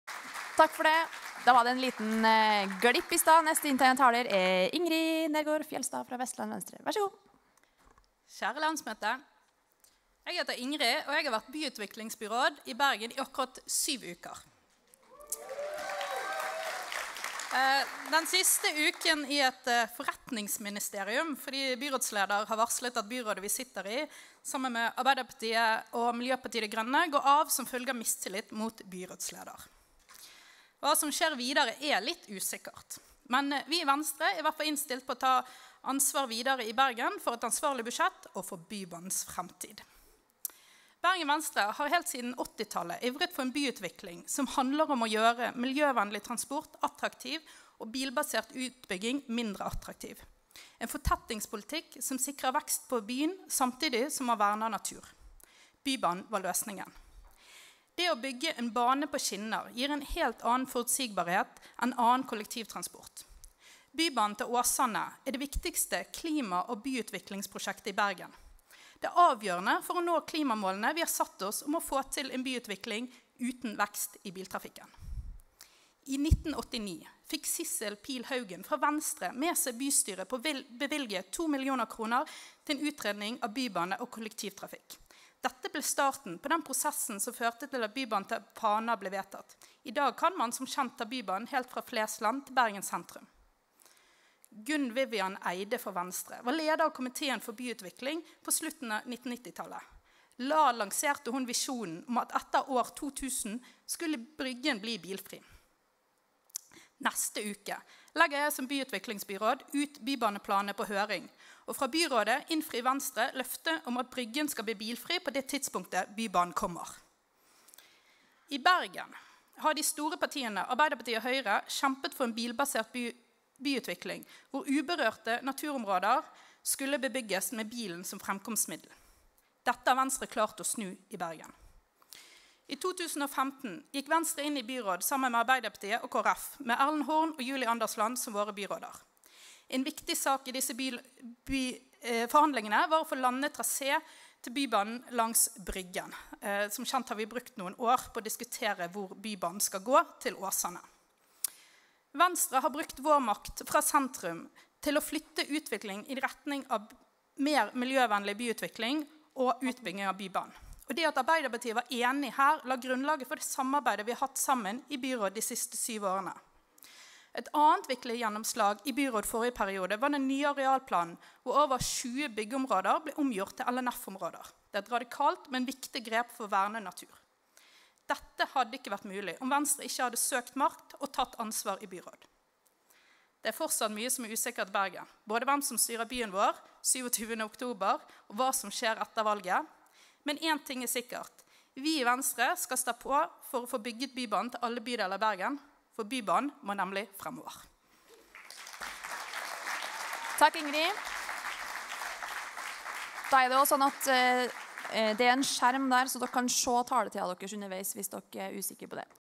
Ingrid Nergaard Fjeldstad (Vestland Venstre) - Venstres landsmøte 2022 (GPD)
Innlegg i "Generell politisk debatt" på Venstres landsmøte 2022 (Gardermoen) En Venstre-video